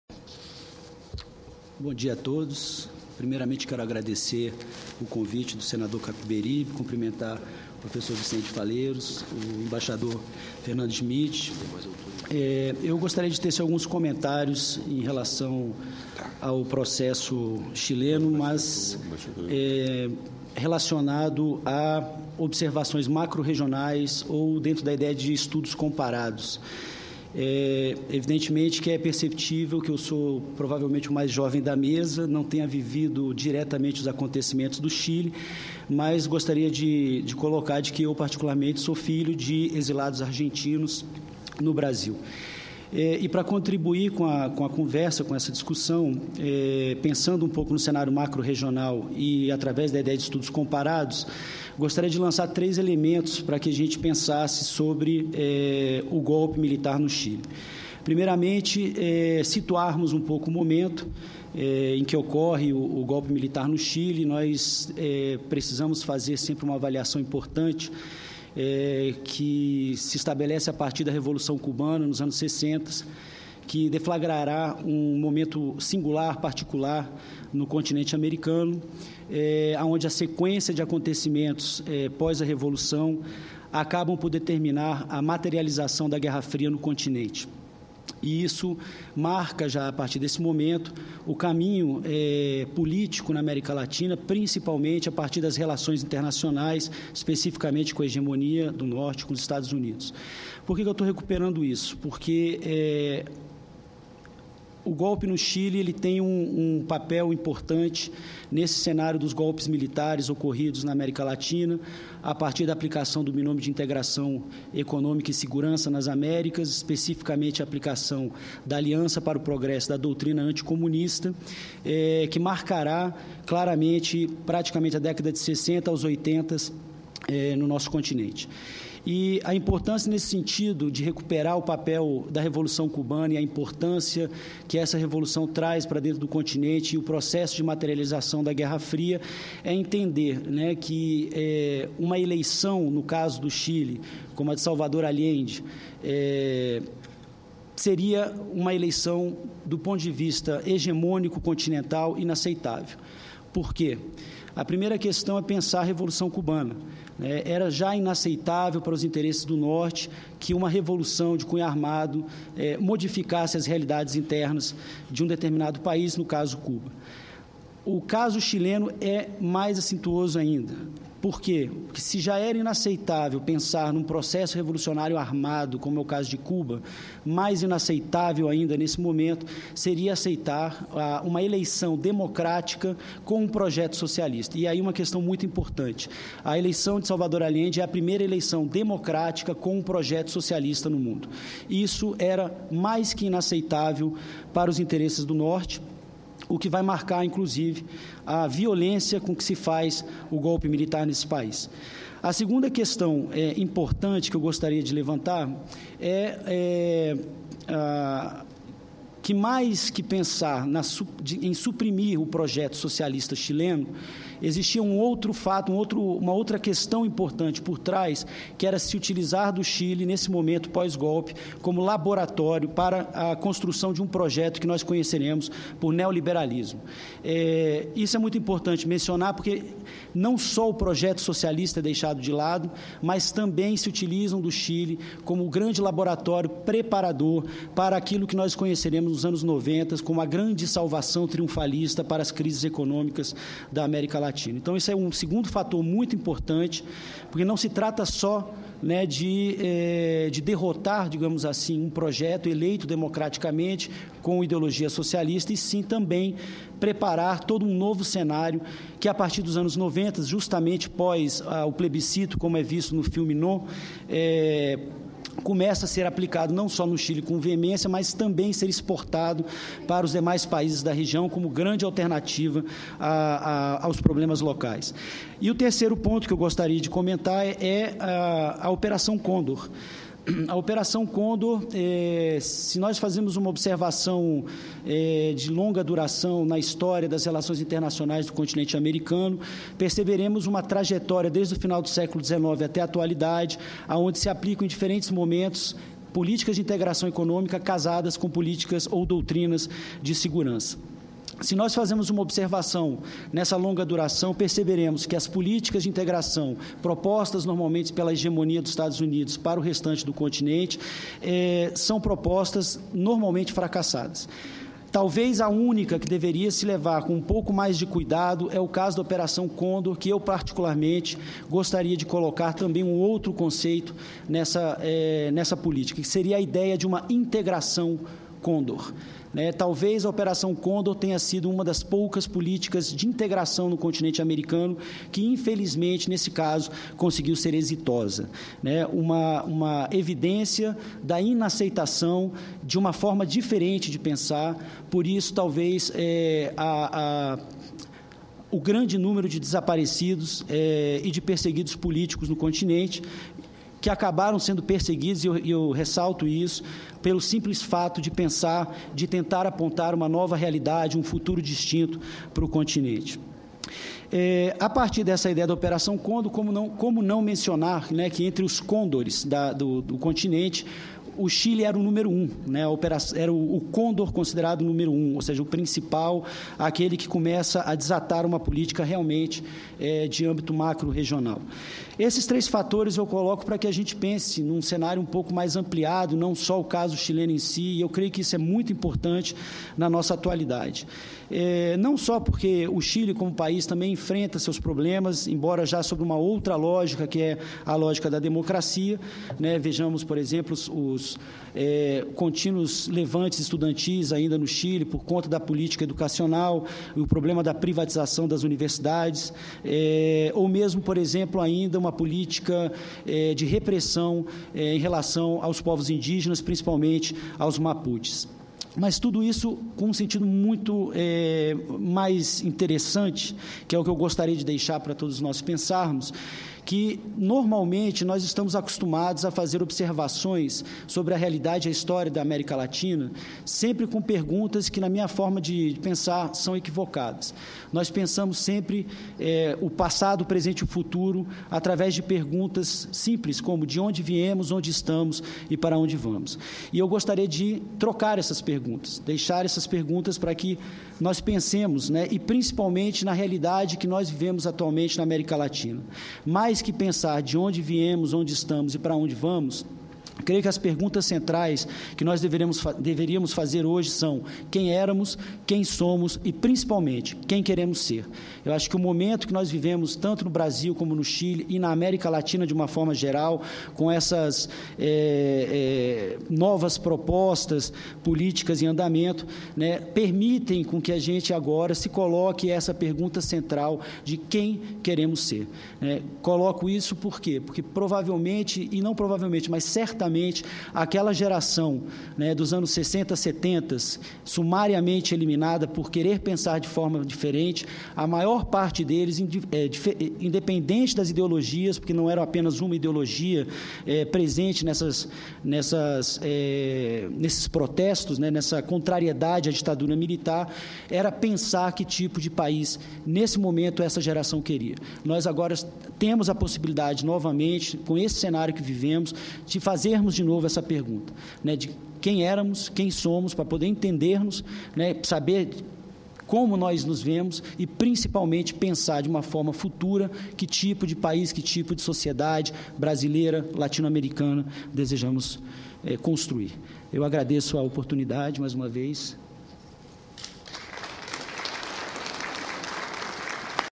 Prof. Jacques de Novion relembra o momento em que ocorreu o golpe militar no Chile
09/09/2013 - Sessão Especial - Prof. Jacques de Novion relembra o momento em que ocorreu o golpe militar no Chile